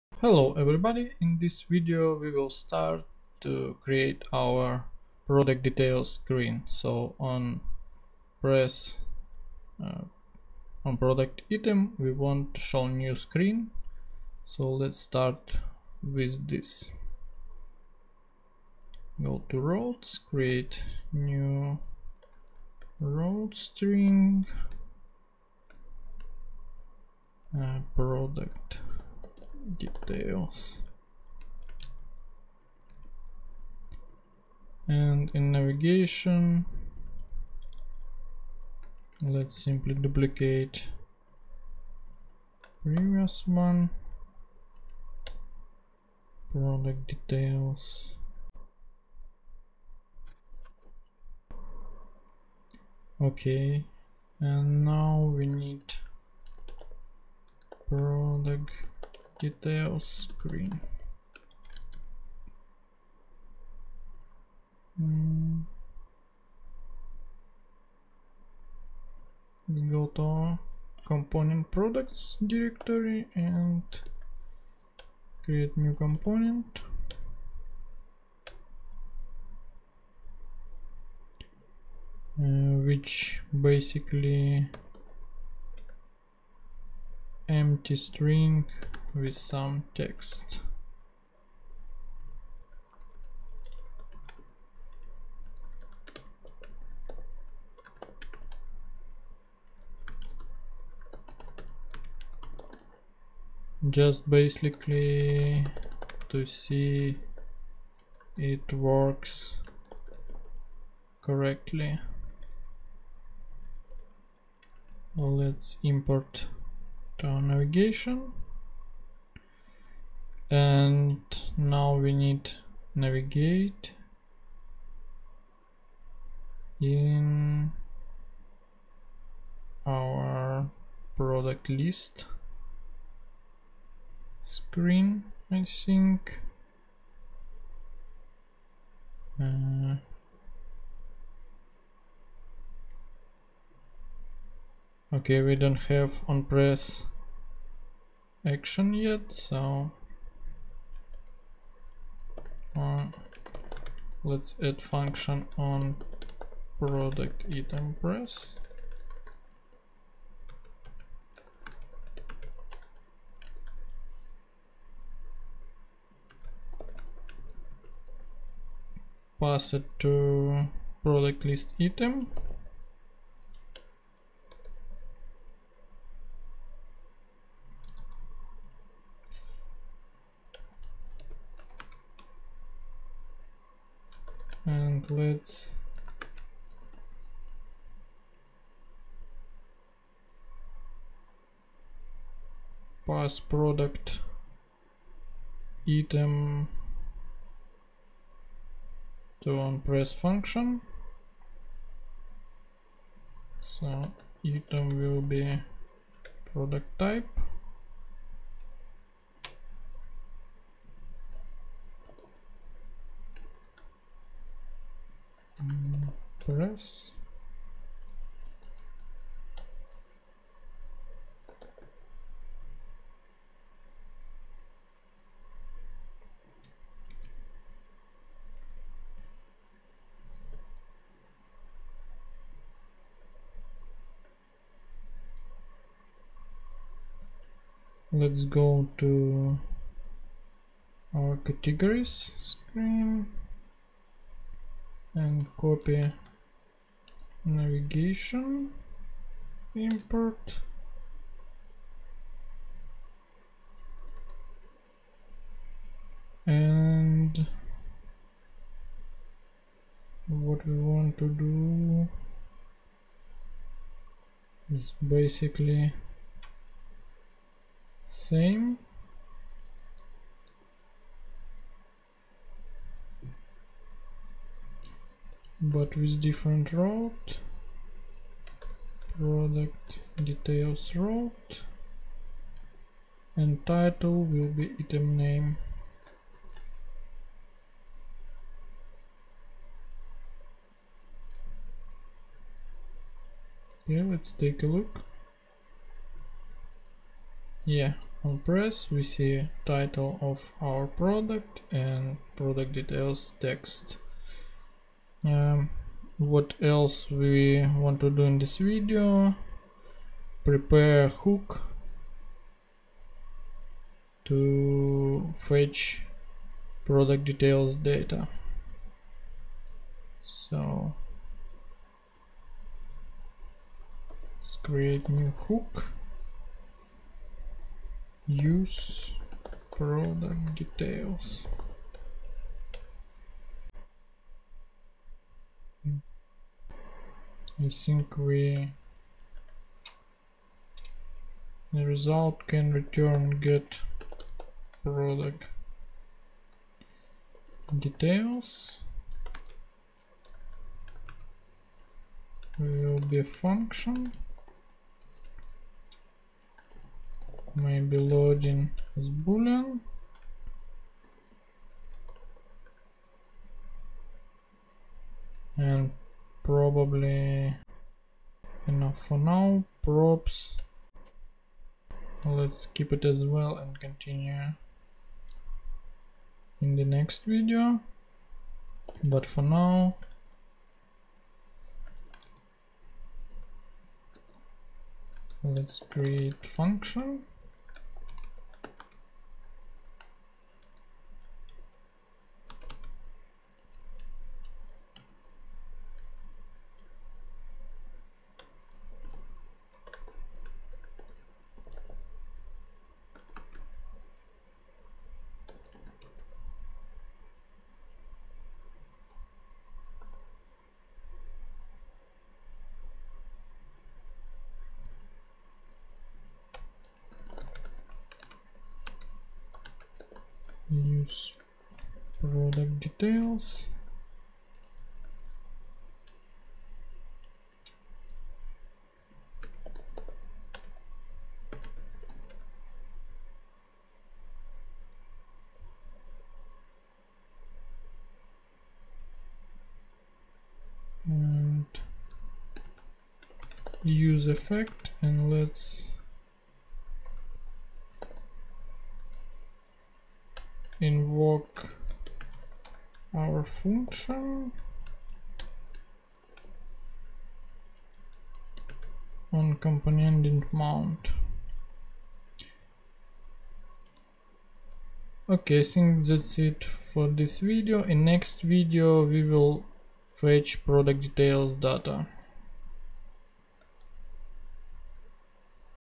#16 - Product Details Screen Part 1 | React Native open-source eCommerce App
In this video, we will prepare a template screen for our Product Details.

This video series about how I'm building a React Native open-source mobile eCommerce application connected to the Magento 2 backend. It will cover different topics in the development process.